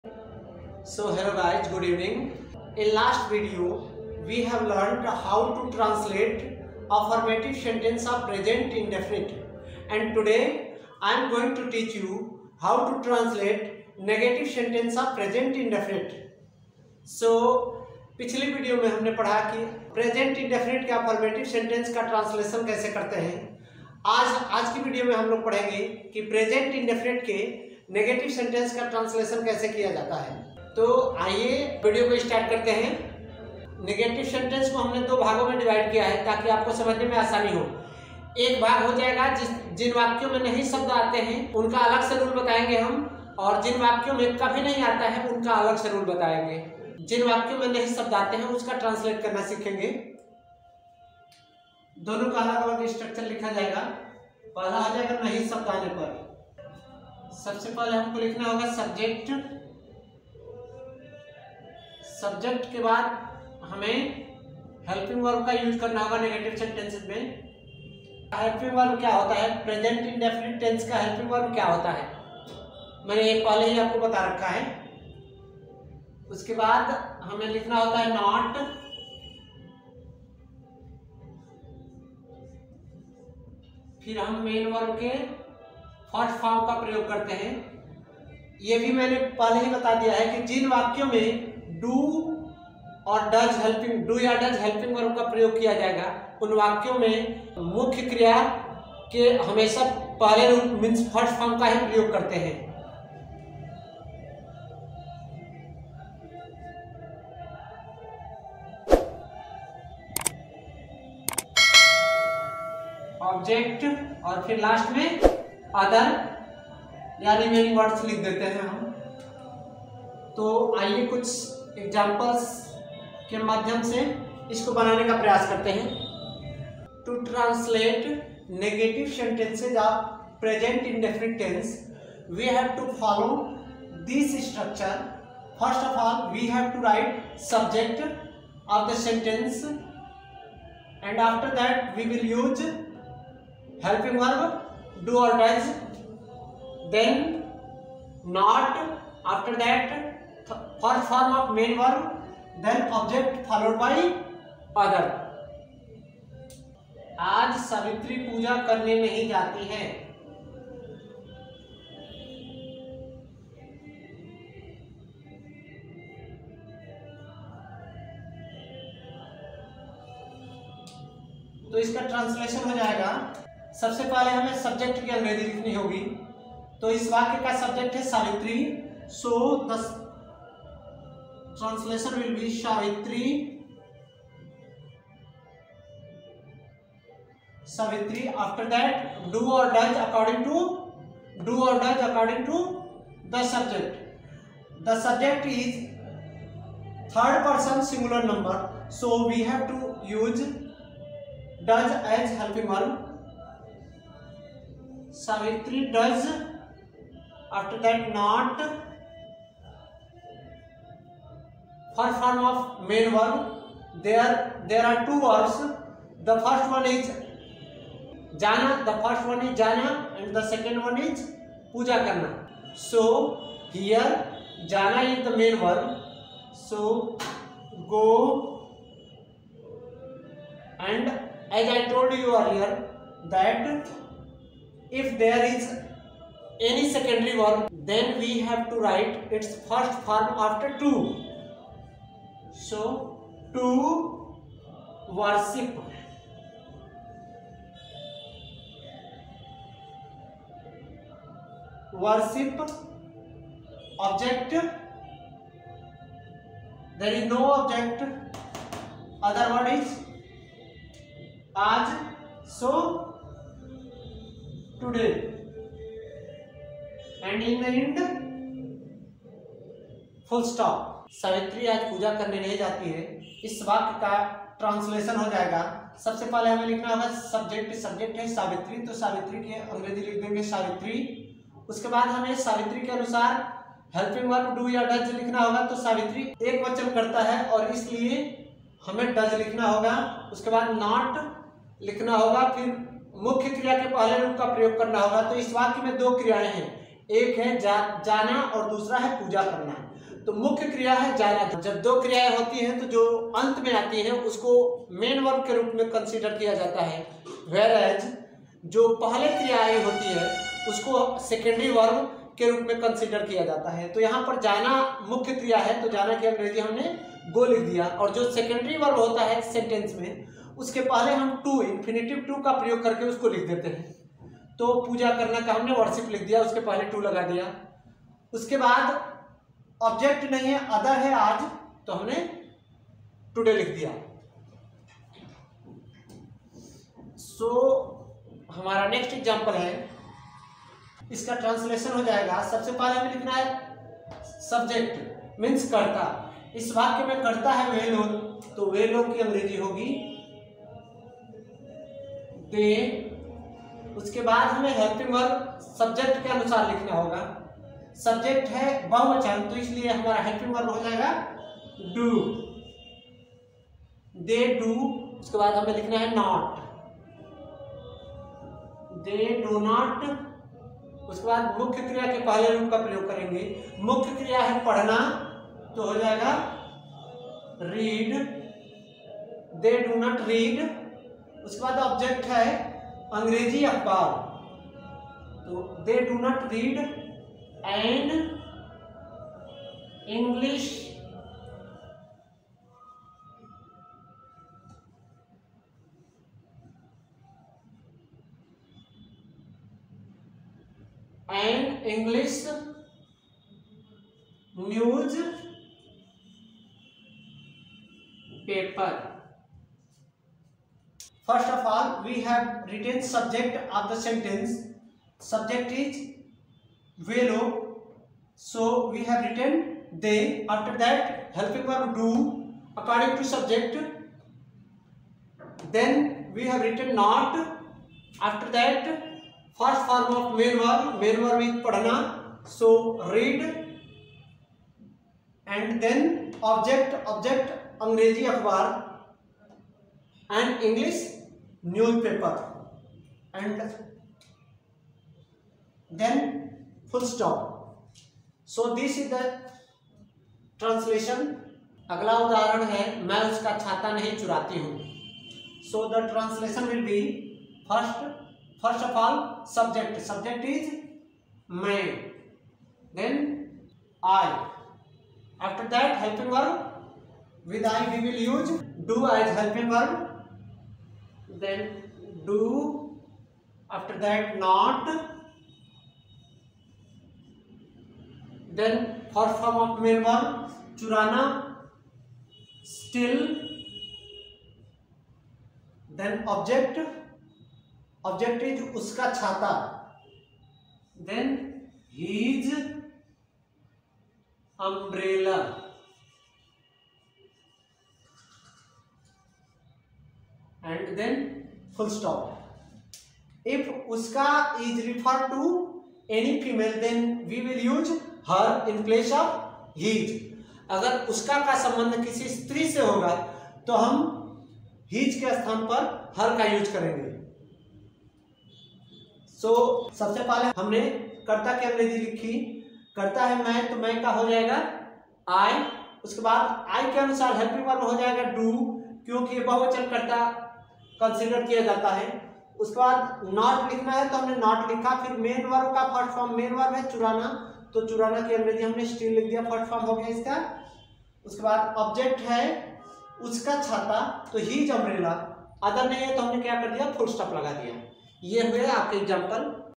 Tense🎓 | Tense सीखें आसानी से 👑 | learn tense🔰| tenseसीखें बिल्कुल basic से🙏| learn tense in english
Dosto is video me humne tense ke present indefinite tense ke negative sentences ka english translation sikhaya hai.